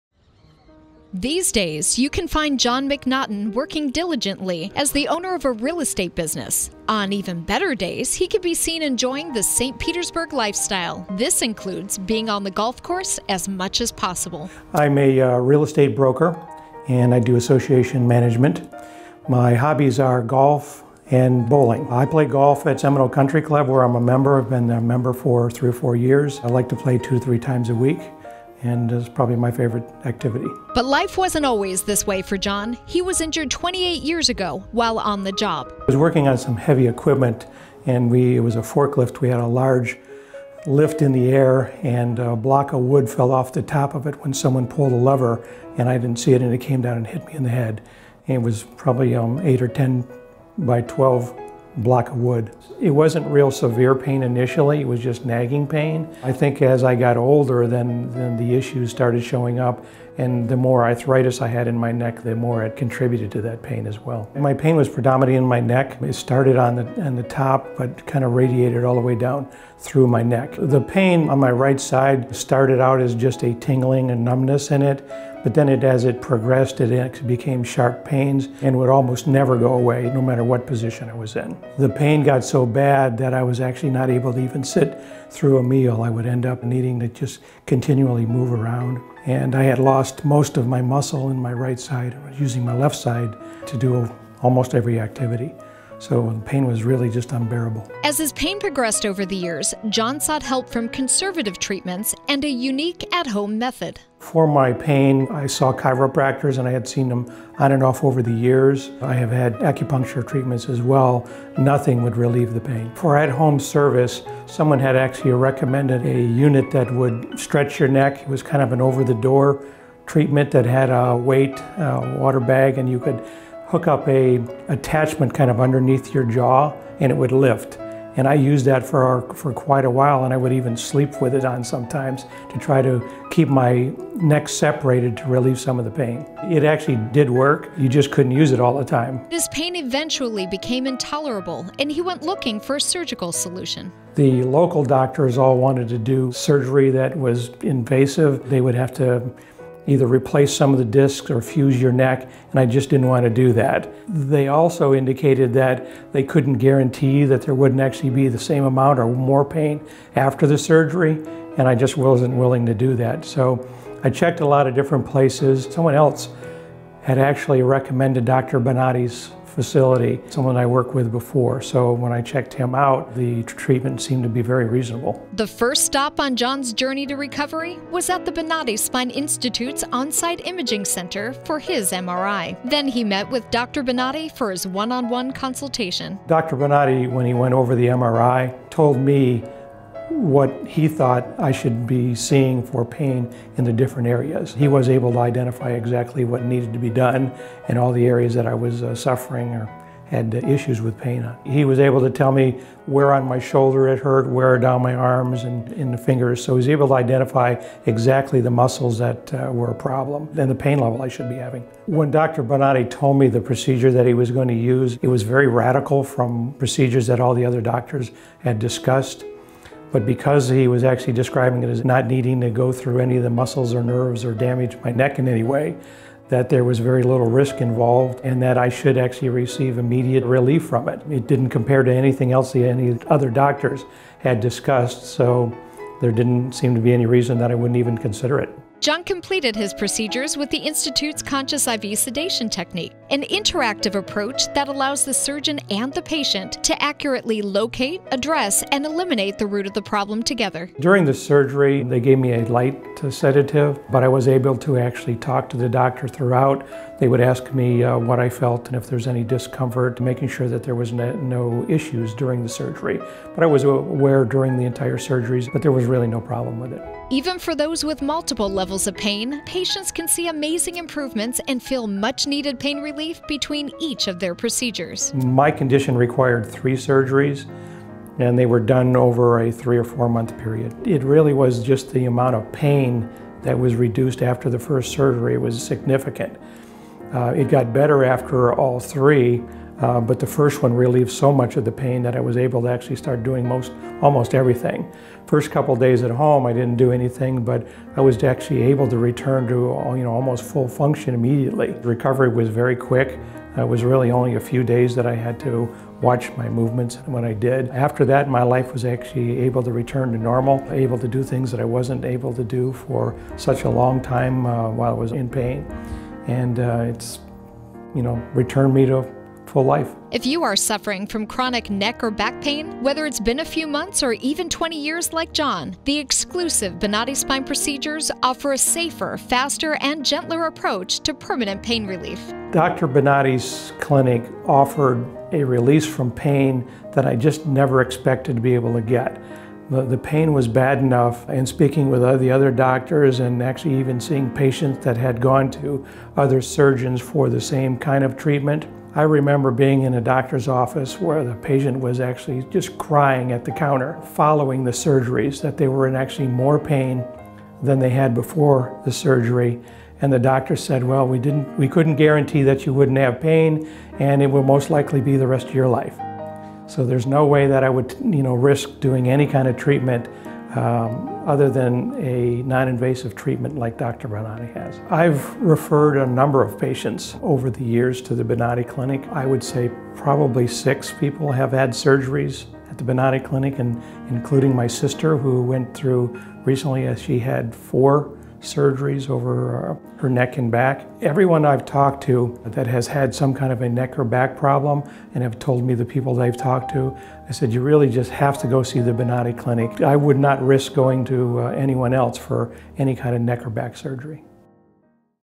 Back to Life: John McNaughton
Even after 20 years, the Bonati Spine Procedures are allowing John to live an active, pain-free life.

If you would like to know if you are a candidate for the Bonati Spine Procedures, please or call .